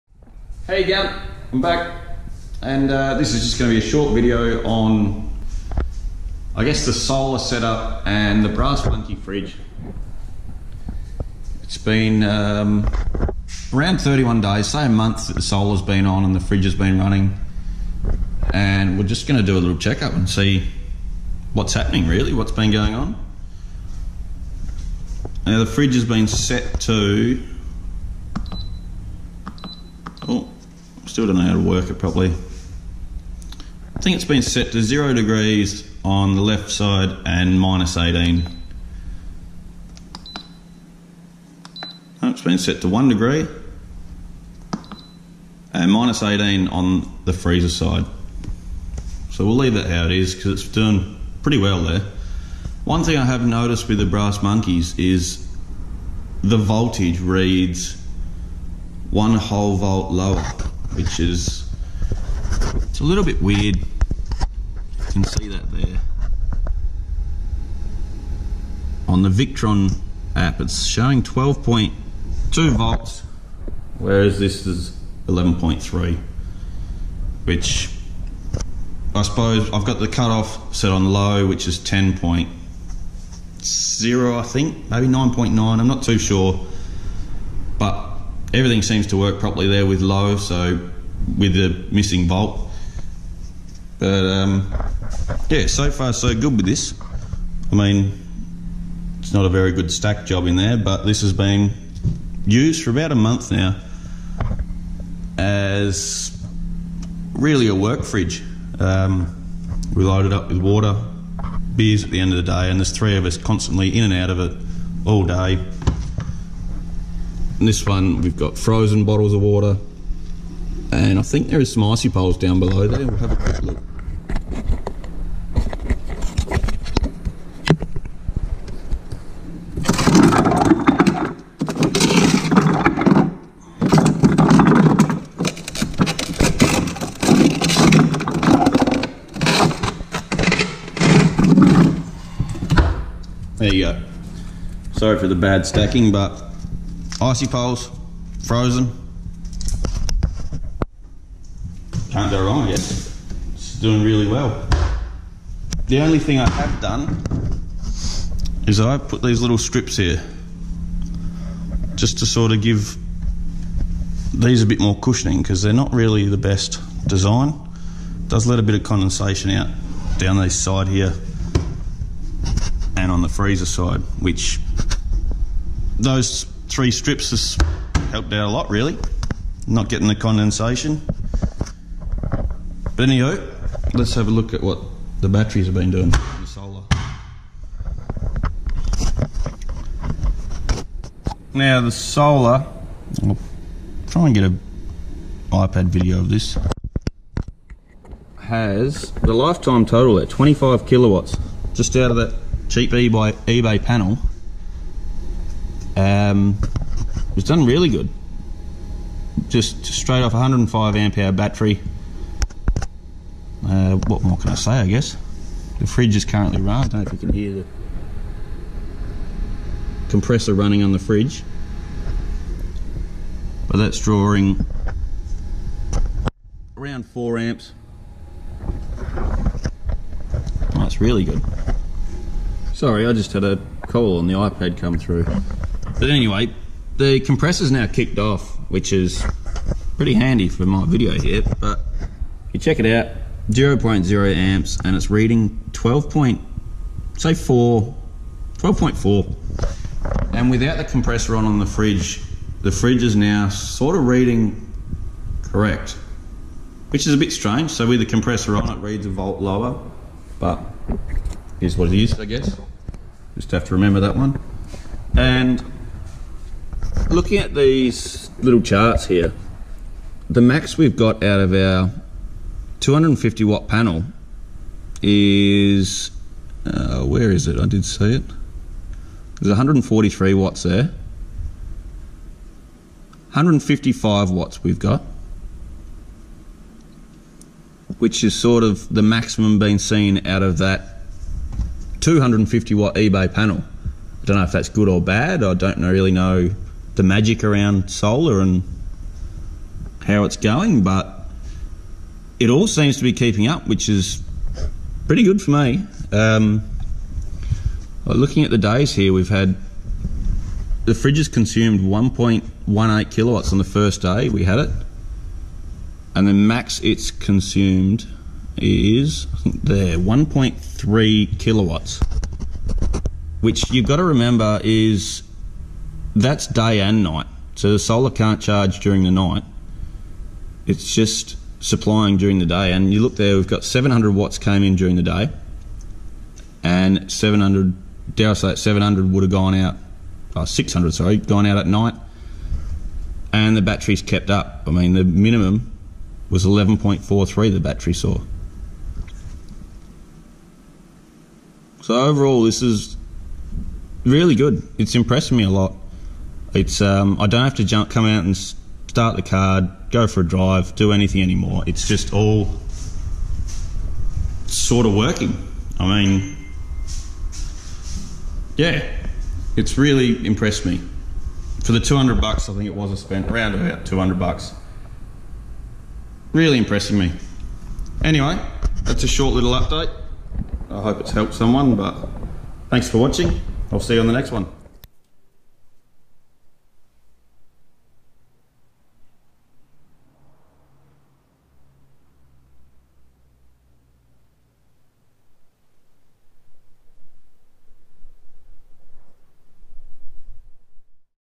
Brass Monkey and Solar Update
Quick update on how my 12v Brass Monkey 95L fridge/freezer and solar setup is going in the back of the Prado. Everything seem to be working as planned and so far can’t fault the fridge for $799